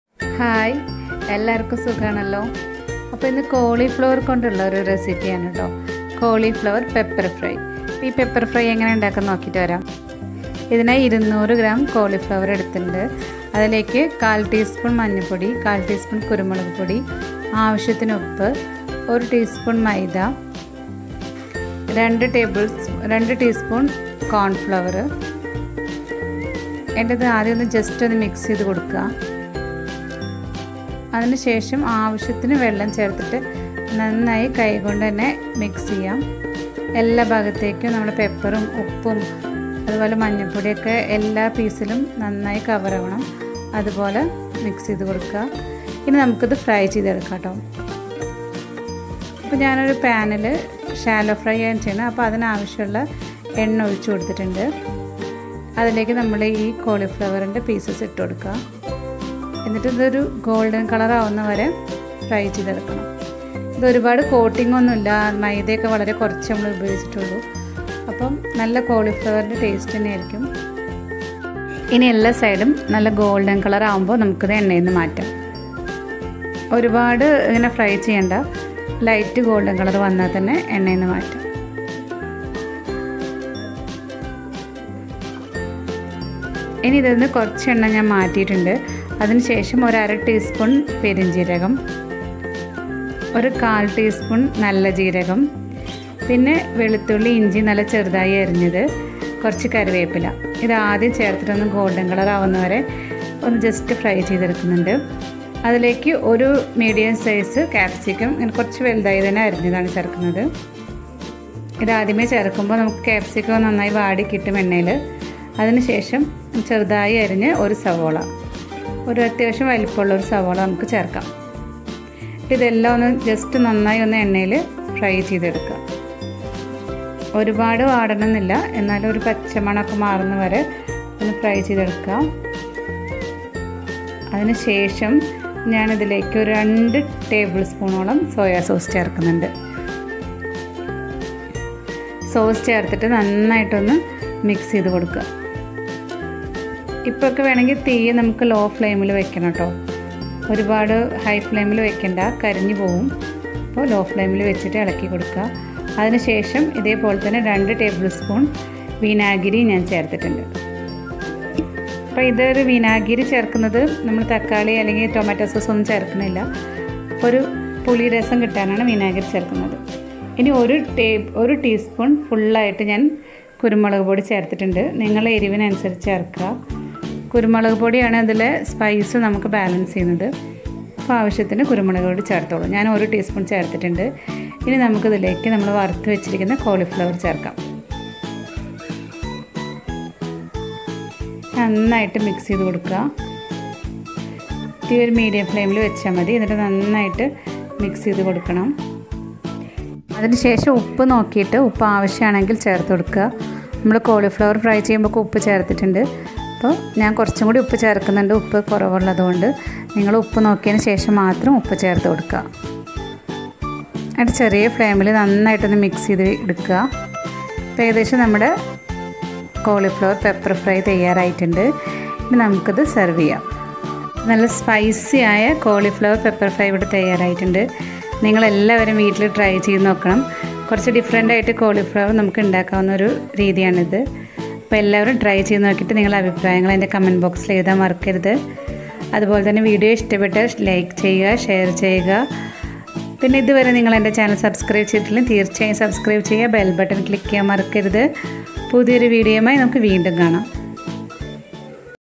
Variety Cauliflower Recipe///Cauliflower Pepper Fry///കോളിഫ്ലവർ പെപ്പർ ഫ്രൈ
Hello Friends .. Please post your comments and feedback in the comment box.

Ingredients:-

Cauliflower : 200g
Turmeric Powder : 1/4tsp
Pepper Powder : 1/4tsp
Salt : To Taste
Maida : 1tsp
Cornflour : 2tsp
Fennel Seeds : 1/2tsp
Cumin Seeds : 1/4tsp
Chopped Ginger & Garlic : 2tbs
Curry Leaves
Capsicum : 1
Onion : 1
Soya Sauce : 2tbs
Vineger : 2tbs
Pepper Powder : 1tsp
Oil